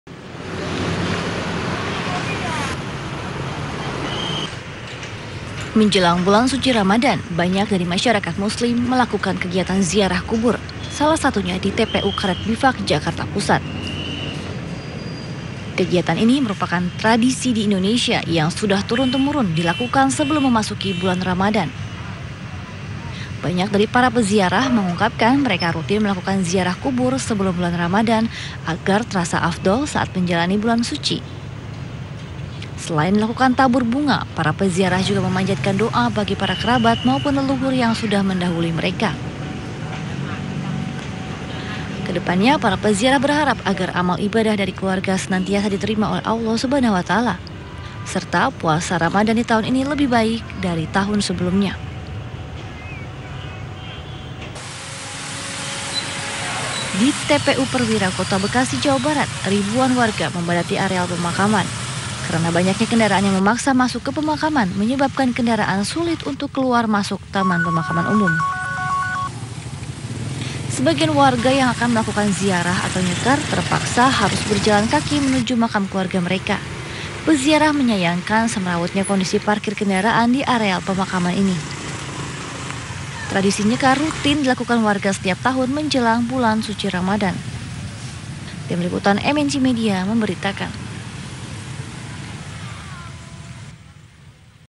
Jelang Puasa Warga Lakukan Ziarah Kubur
Jelang datangnya bulan suci ramadhan, warga mulai melakukan tradisi yang tidak pernah lepas dari datangnya bulan puasa.
Tradisi tersebut adalah ziarah kubur untuk mendoakan keluarga yang sudah mendahului.